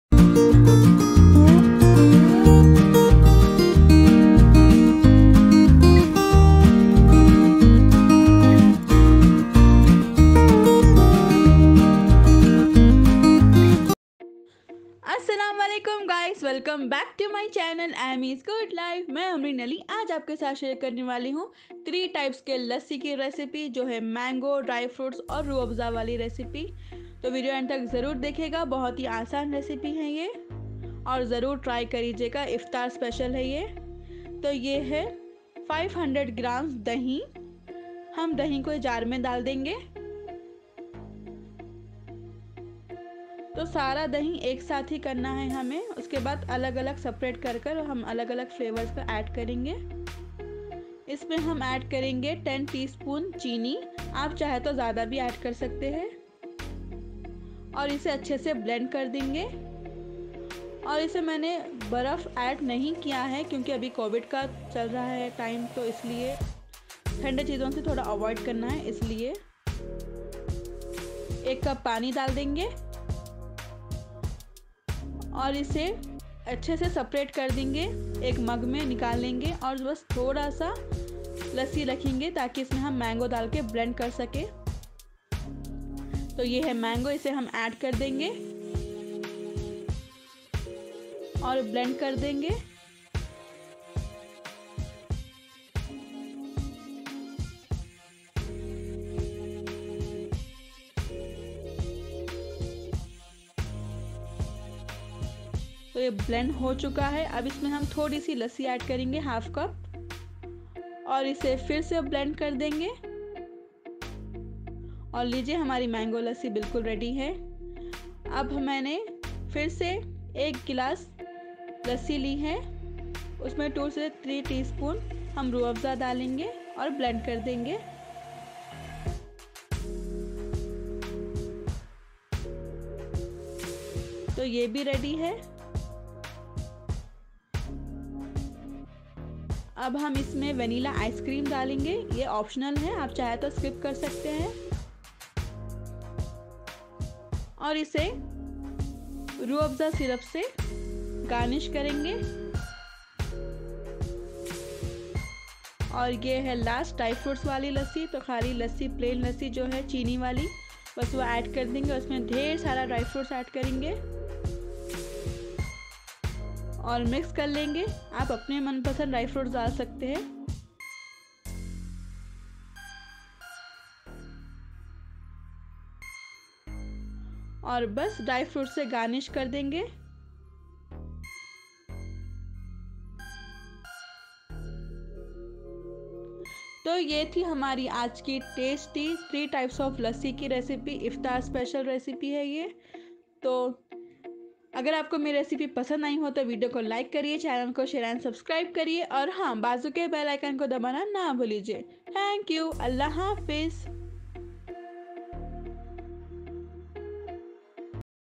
Ramzan Iftar Special 3 Types Of Lassi | Mango, Dry Fruit And Rooh Afza Lassi Recipe| By Amrin Ali
keep watching:)

INGREDIENTS:-
Sugar 10 tsp (else you can add according to your taste)
Vanilla ice cream 1 scoop
Water 1 cup
Mango diced 1/2 cup
Rooh afza syrup 2-3 tsp
Dry fruits (as per your choice)

free editing:- openshot video editor.